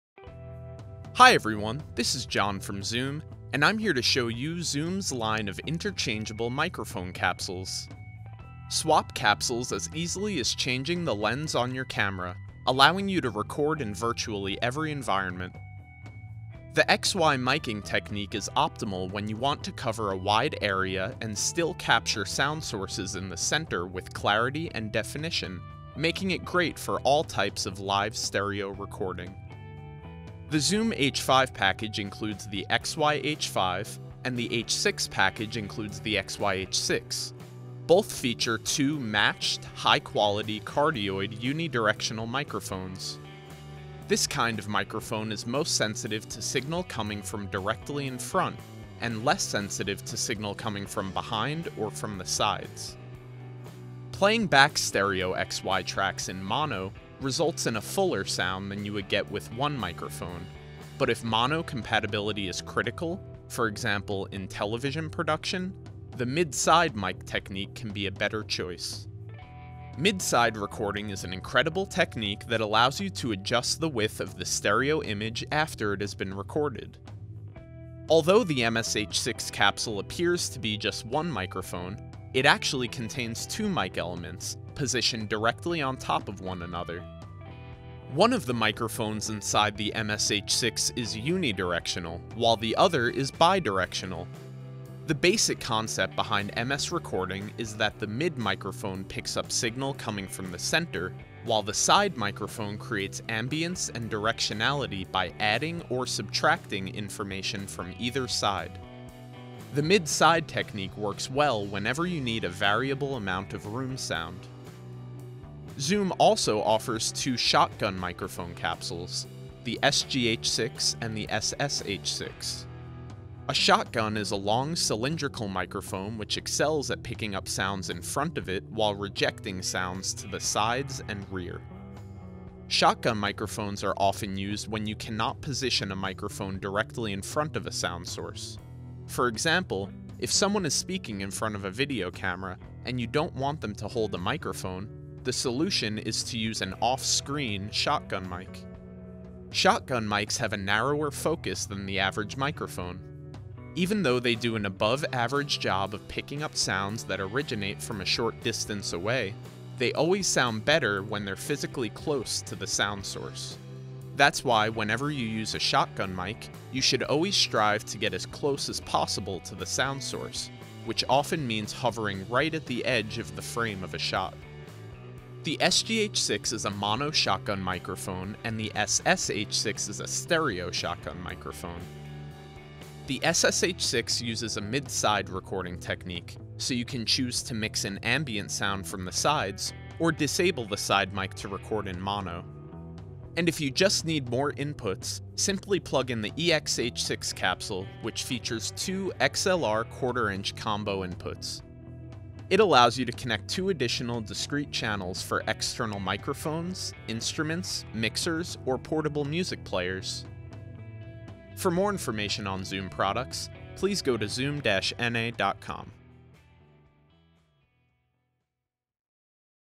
Zoom Mic Capsule Overview
Hi everyone, this is John from Zoom, and I’m here to show you Zoom’s line of interchangeable microphone capsules. Swap capsules as easily as changing the lens on your camera, allowing you to record in virtually every environment.

The X/Y miking technique is optimal when you want to cover a wide area and still capture sound sources in the center with clarity and definition, making it great for all types of live stereo recording. The Zoom H5 package includes the XYH-5 and the H6 package includes the XYH-6. Both feature two matched high-quality cardioid unidirectional microphones. This kind of microphone is most sensitive to signal coming from directly in front, and less sensitive to signal coming from behind or from the sides.

Playing back stereo X/Y tracks in mono results in a fuller sound than you would get with one microphone, but if mono compatibility is critical (for example, in television production), the Mid-Side (MS) mic technique can be a better choice.

Mid-Side recording is an incredible technique that allows you to adjust the width of the stereo image after it has been recorded. Although the MSH-6 MS capsule appears to be just one microphone, it actually contains two mic elements, positioned directly on top of one another. One of the microphones inside the MSH-6 is unidirectional, while the other is bi-directional.
The basic concept behind MS recording is that the Mid microphone picks up signal coming from the center, while the Side microphone creates ambience and directionality by adding or subtracting information from either side. The Mid-Side technique works well whenever you need a variable amount of room sound.

Zoom also offers two shotgun microphone capsules: the SGH-6 and the SSH-6. A “shotgun” is a long, cylindrical microphone which excels at picking up sounds in front of it while rejecting sounds to the sides and rear. Shotgun microphones are often used when you can not position a microphone directly in front of a sound source. For example, if someone is speaking in front of a video camera and you don’t want them to hold a microphone, the solution is to use an off-screen shotgun mic.

Shotgun mics have a narrower focus than the average microphone. Even though they do an above-average job of picking up sounds that originate from a short distance away, they always sound better when they’re physically close to the sound source. That’s why whenever you use a shotgun mic, you should always strive to get as close as possible to the sound source, which often means hovering right at the edge of the frame of a shot.

The SGH-6 is a mono shotgun microphone and the SSH-6 is a stereo shotgun microphone. The SSH-6 uses a Mid-Side recording technique, so you can choose to mix in ambient sound from the sides, or disable the side mic to record in mono.

If you just need more inputs, simply plug in the EXH-6 capsule which features two XLR ¼” combo inputs. It allows you to connect two additional discrete channels for external microphones, instruments, mixers or portable music players.

For more information on Zoom products, please go to zoom-na.com